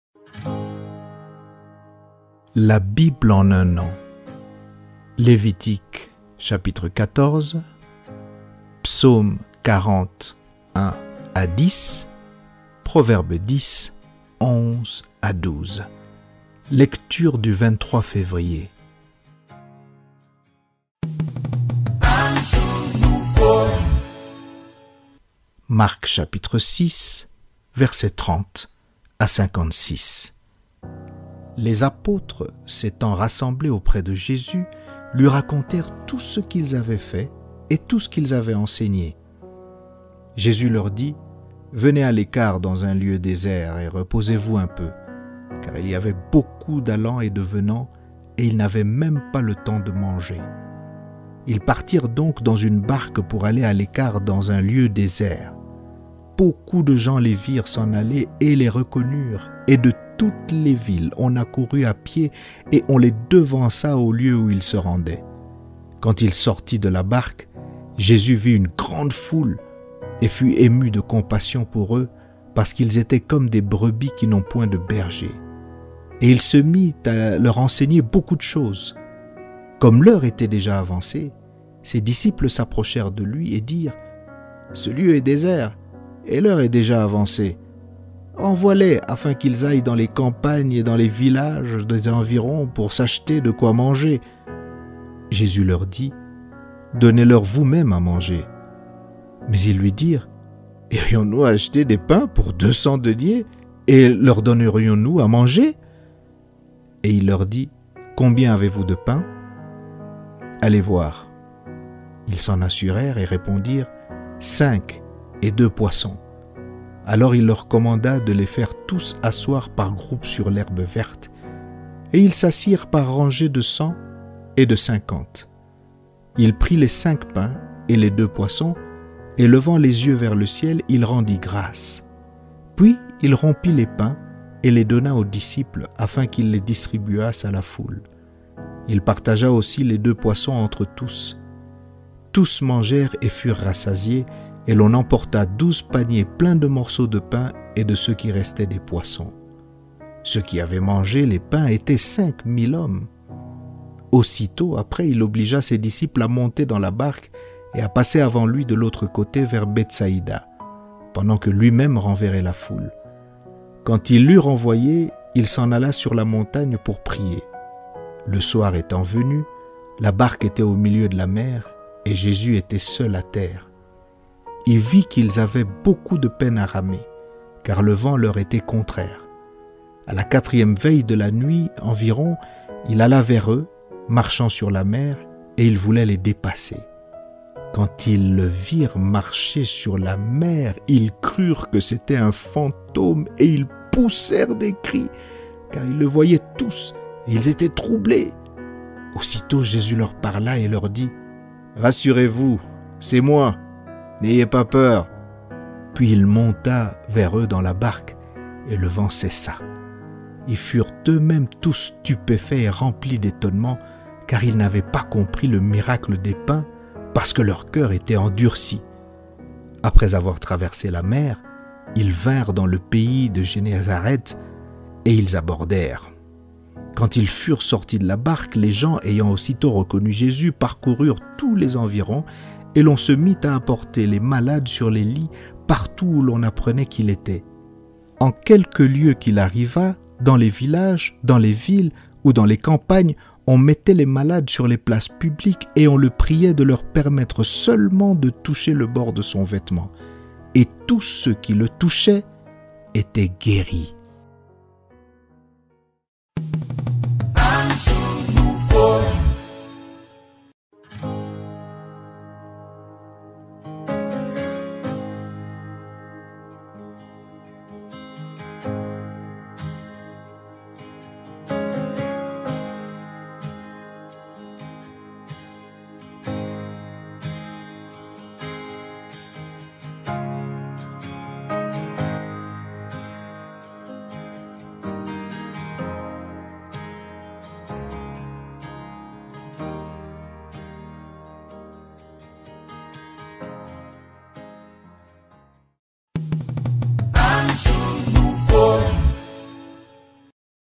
La bible en un an | Lecture du 23 février
-Extraits-
Proverbes 10:10-11
Pv 10,11 La bouche du juste est une source de vie, Mais la violence couvre la bouche des méchants.
Pv 10,12 La haine excite des querelles, Mais l'amour couvre toutes les fautes.
De Psaumes 40:1-10
V5,6,8,9
Ps 40,5 Heureux l'homme qui place en l'Eternel sa confiance, Et qui ne se tourne pas vers les hautains et les menteurs!
Ps 40,6 Tu as multiplié, Eternel, mon Dieu! Tes merveilles et tes desseins en notre faveur; Nul n'est comparable à toi; Je voudrais les publier et les proclamer, Mais leur nombre est trop grand pour que je les raconte.
Ps 40,8 Alors je dis: Voici, je viens Avec le rouleau du livre écrit pour moi.
Ps 40,9 Je veux faire ta volonté, mon Dieu! Et ta loi est au fond de mon coeur..
De Marc 6:30-56
V34,41,42,48,56
Mc 6,34 Quand il sortit de la barque, Jésus vit une grande foule, et fut ému de compassion pour eux, parce qu'ils étaient comme des brebis qui n'ont point de berger; et il se mit à leur enseigner beaucoup de choses.
Mc 6,41 Il prit les cinq pains et les deux poissons et, levant les yeux vers le ciel, il rendit grâces. Puis, il rompit les pains, et les donna aux disciples, afin qu'ils les distribuassent à la foule. Il partagea aussi les deux poissons entre tous.
Mc 6,42 Tous mangèrent et furent rassasiés,
Mc 6,48 Il vit qu'ils avaient beaucoup de peine à ramer; car le vent leur était contraire. A la quatrième veille de la nuit environ, il alla vers eux, marchant sur la mer, et il voulait les dépasser.
Mc 6,56 En quelque lieu qu'il arrivât, dans les villages, dans les villes ou dans les campagnes, on mettait les malades sur les places publiques, et on le priait de leur permettre seulement de toucher le bord de son vêtement. Et tous ceux qui le touchaient étaient guéris.
De Lévitique 14:1-57
V54-57
Lév 14,54 Telle est la loi pour toute plaie de lèpre et pour la teigne,
Lév 14,55 pour la lèpre des vêtements et des maisons,
Lév 14,56 pour les tumeurs, les dartres et les taches:
Lév 14,57 elle enseigne quand une chose est impure, et quand elle est pure. Telle est la loi sur la lèpre.